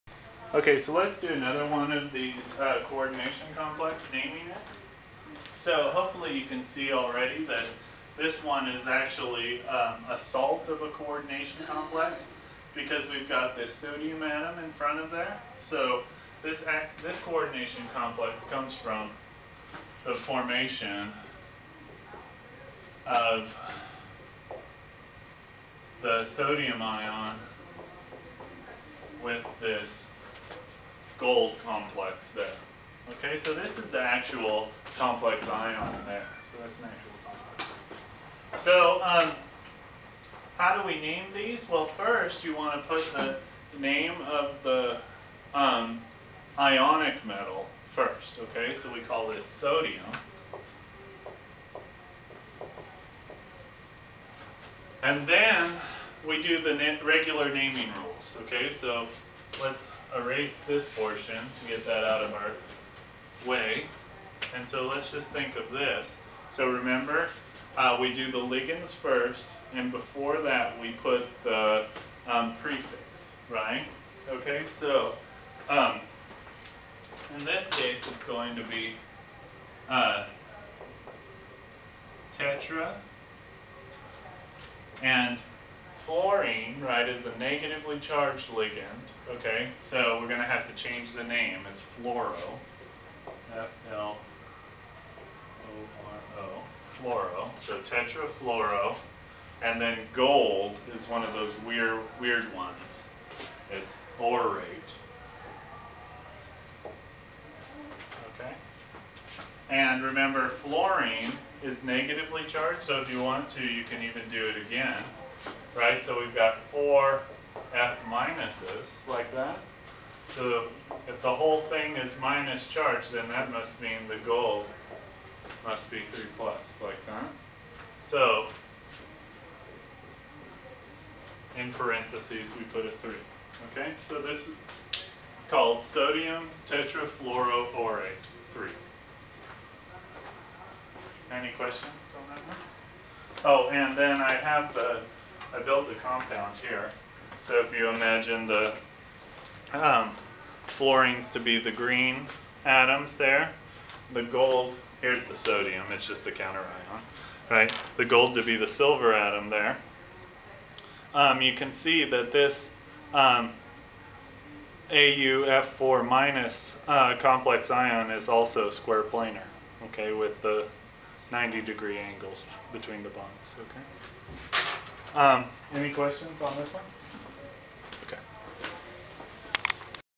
Naming Coordination Complexes 002
Write the systematic name for the coordination complex NaAuF4.